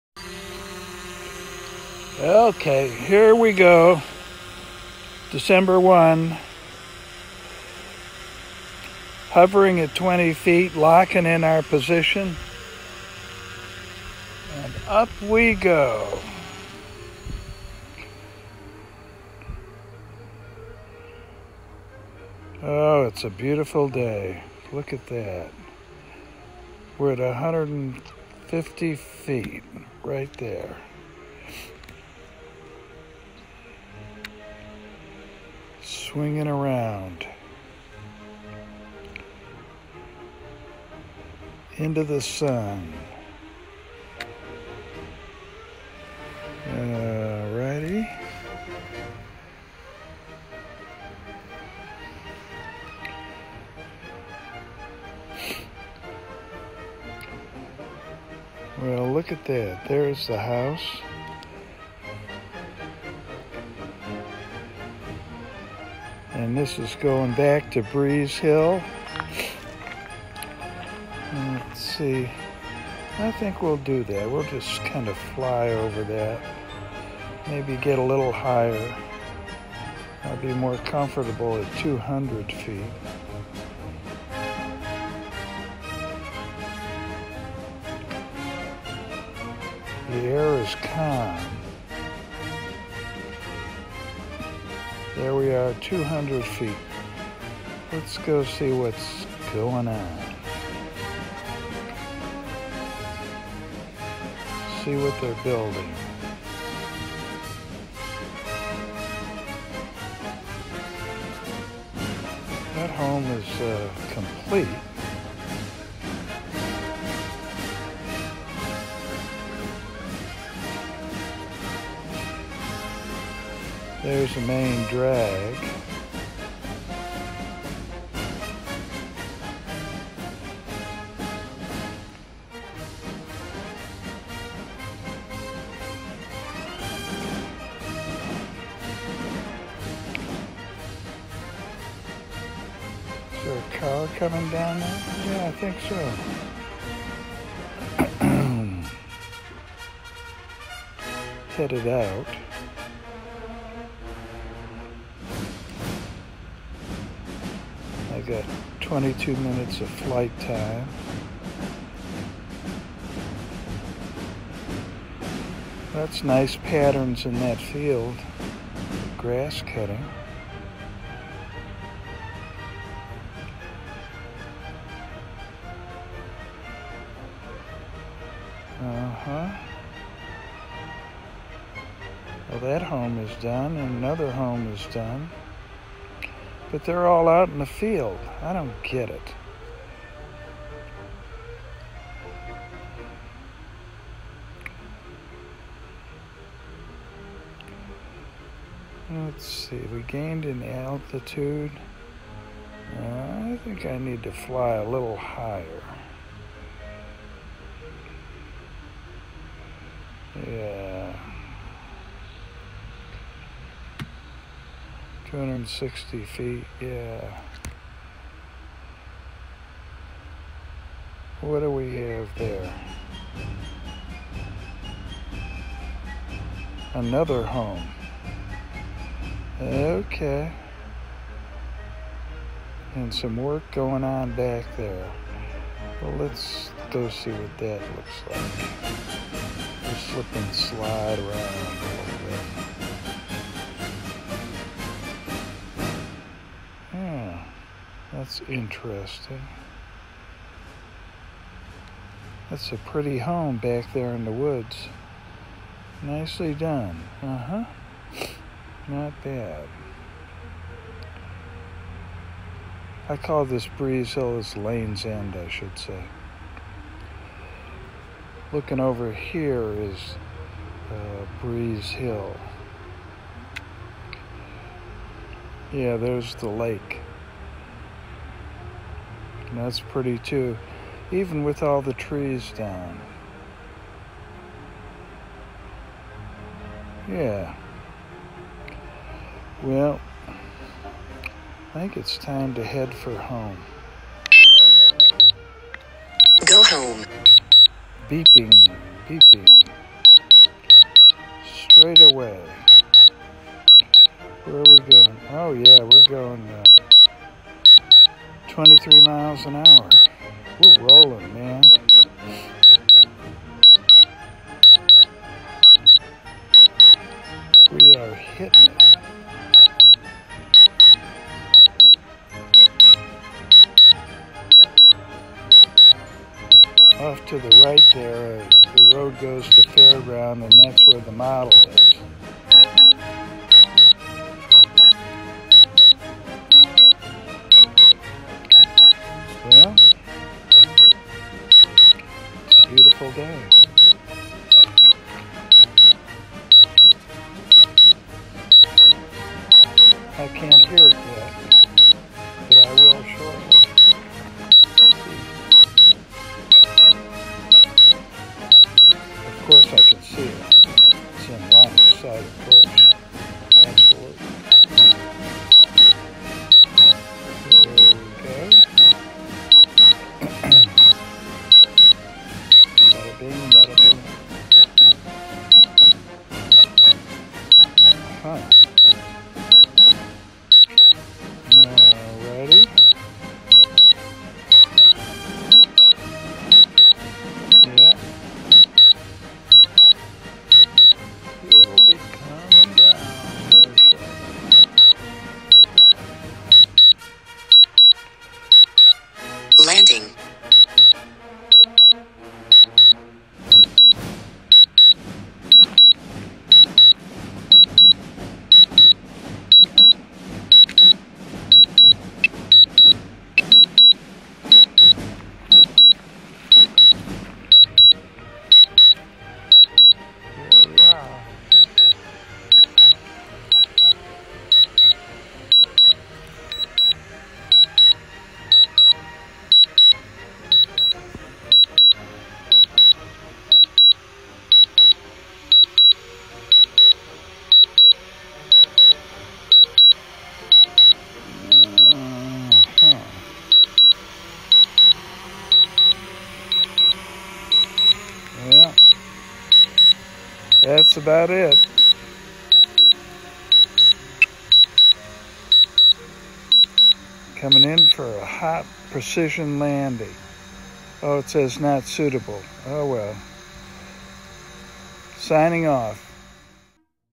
Breaking 100 Miles
I’ve now flown over 100 miles in 365 flights with the DJI Mavic Pro